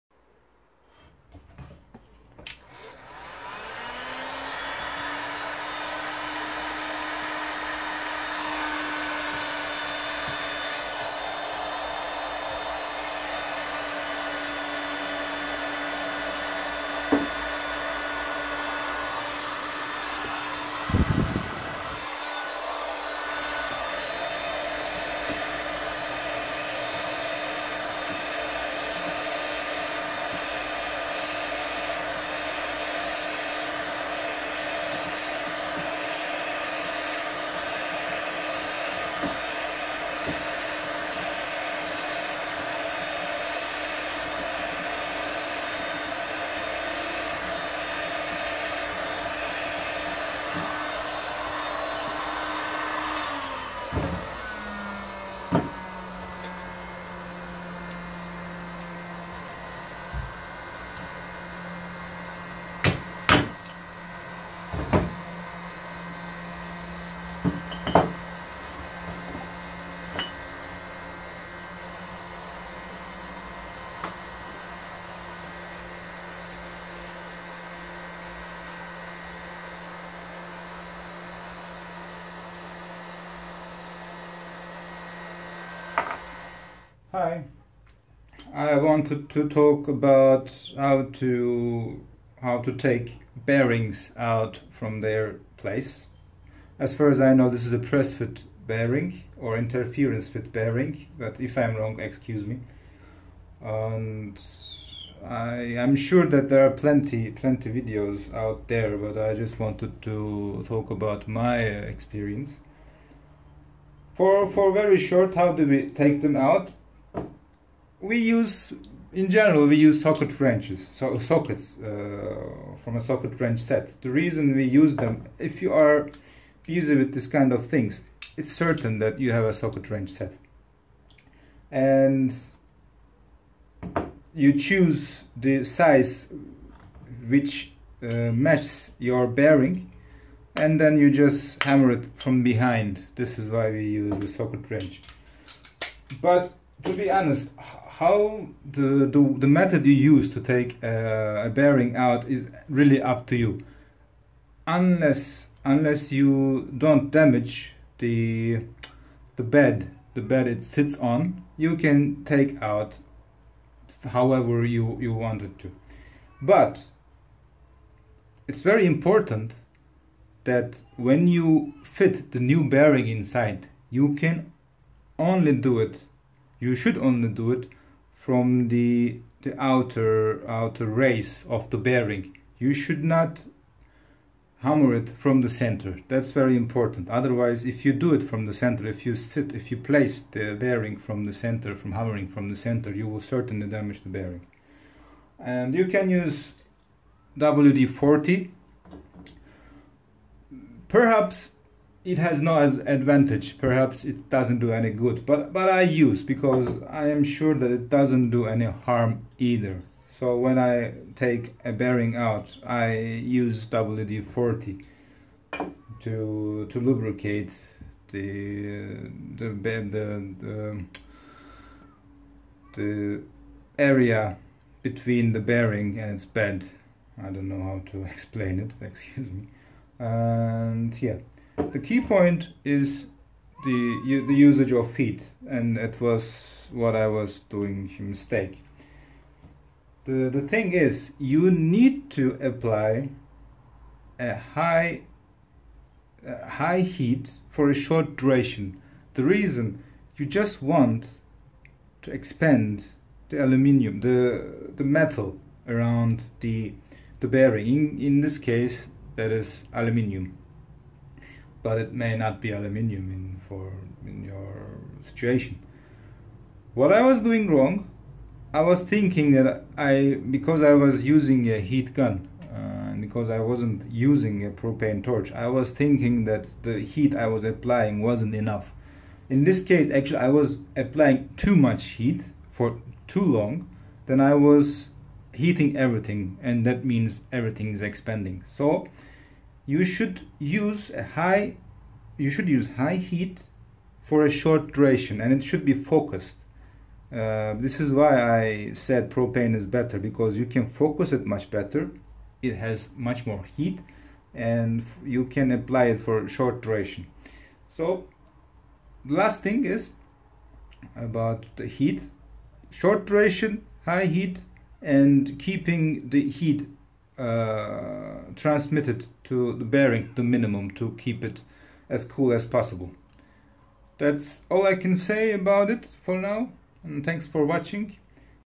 Bearing Removal Tutorial. How to Remove a Bearing. Motorcycle Rear Sprocket
Here my own ideas and experience with bearing removal.